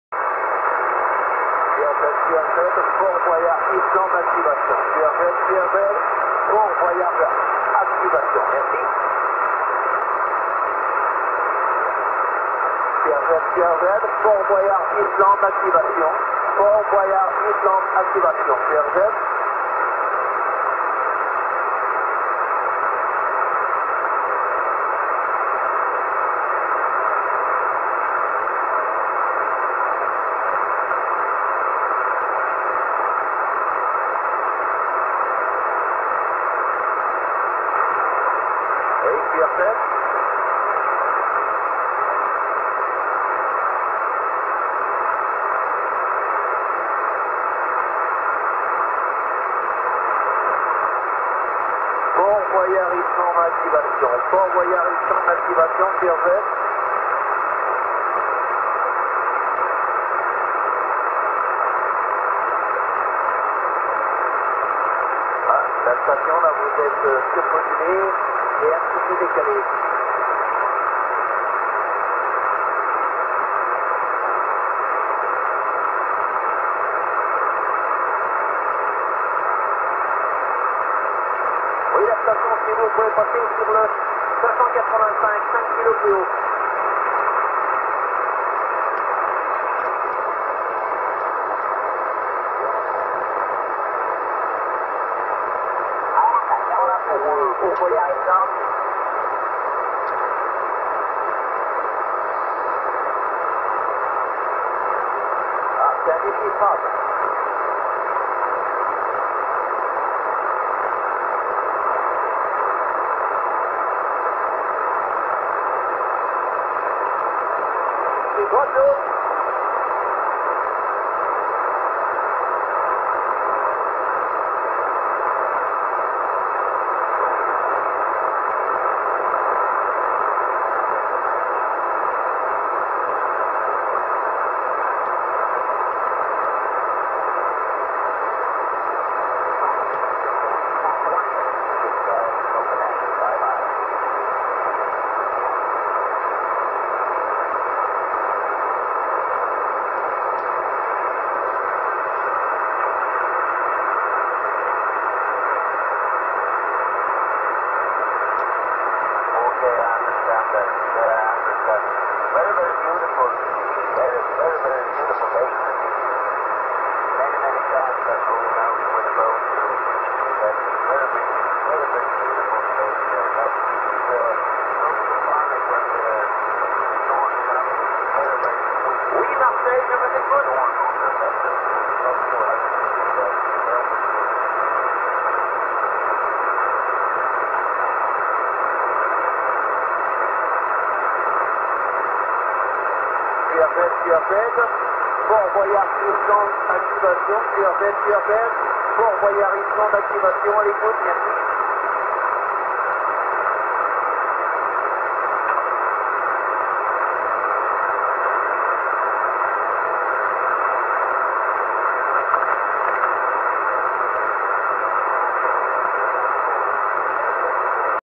Expedition 27 Mhz et RA de Fort Boyard 26 07 2012
- Contest Expédition Cibi / RadioAmateur Fort Boyard en se moment même (25/26/07/2012).
Moi, je suis entre Saint Emilion et Castillon la Bataille (dep.33)
Mon contacte à était confirmer le 26/07 à 9H27). Merci Bernard de la FFUCB.
(la vidéo a était faite aussitôt après ma confirmation, au meilleur moment.)
- Cette vidéo restera dans les annales et deviendra certainement historique pour deux raisons:
1- c'est la dernière fois qu'une expédition radio ce passera la-bas. L'état évoquant des problèmes de sécurité.
2- La personne que l'on entend est tout simplement Mr Bernard Arnut. Un Grand Monsieur qui a toujours défendu les valeurs de la Cibi francophone. Il est également en étroite collaboration avec certain pays comme la Belgique et le Québec.

Dundee33 / (14) D R 33
(vidéo expérimental)